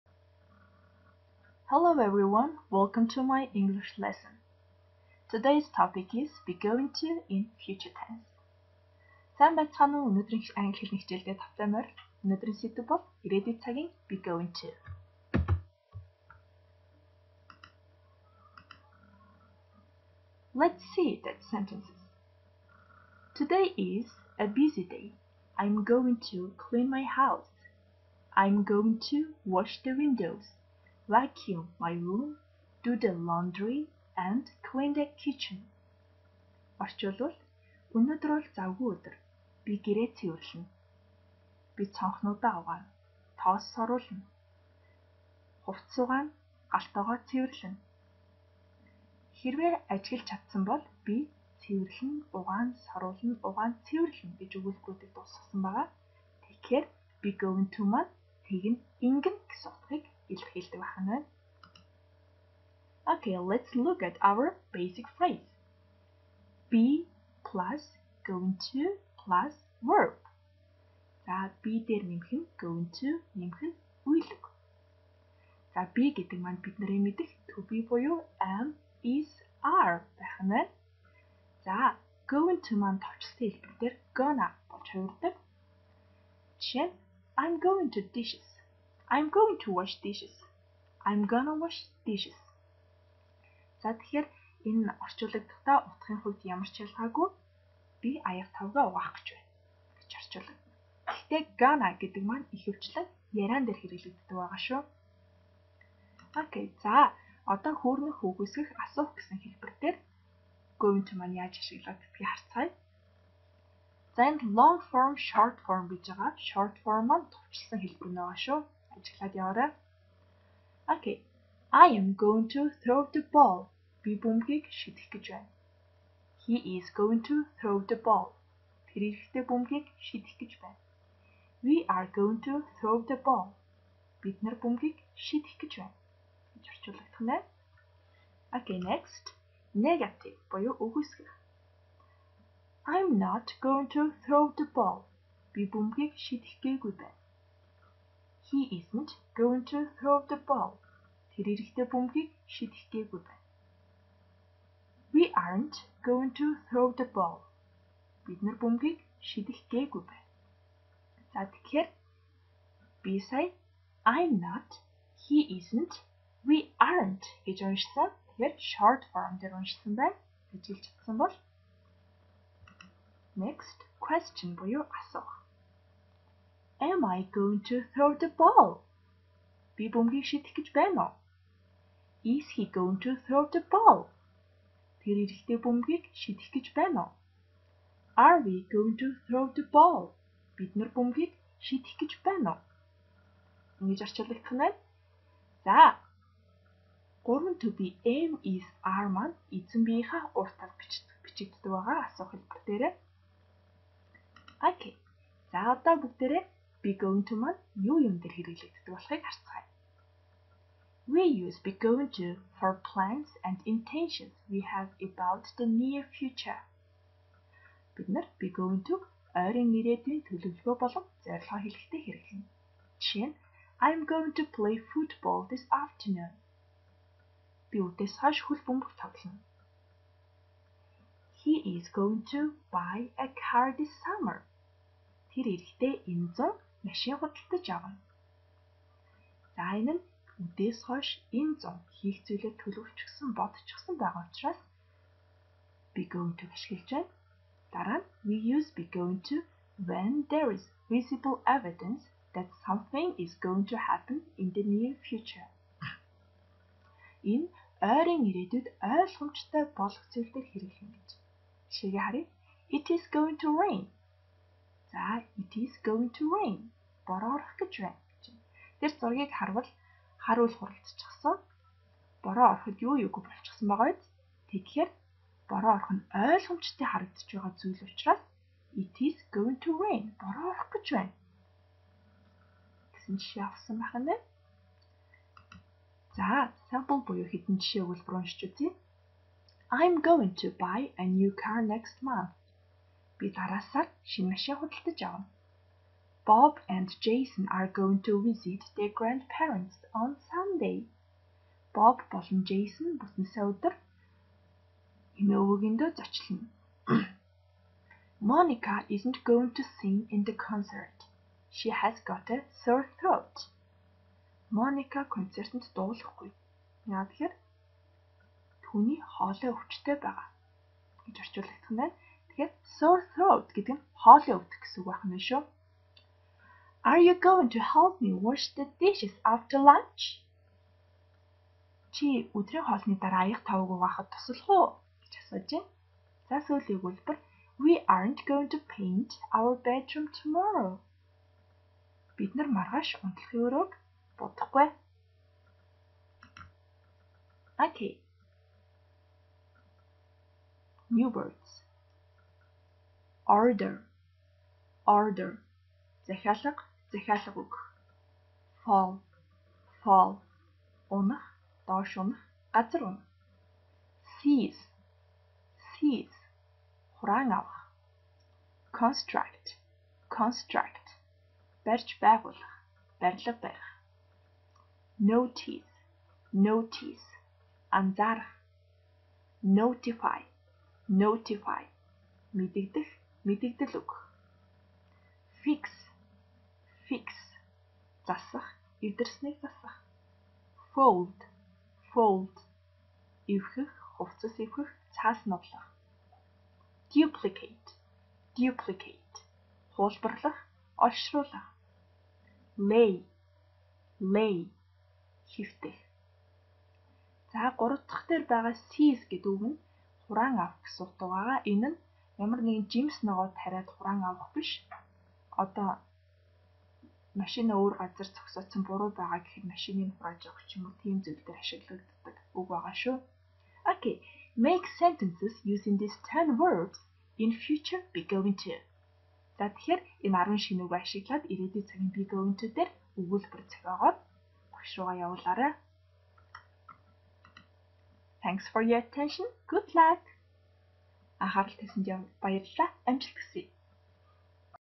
8-р анги Англи хэл. Ирээдүй цагийн "be going to", Багш Г.Гантуяа
Нийслэлийн ерөнхий боловсролын лаборатори 14 дүгээр сургуулийн нээлттэй хичээл, зөвлөмжүүд.
Та бүхэн хичээлтэй холбоотой санал хүсэлтээ сэтгэгдлээр үлдээгээрэй.
Баярлалаа.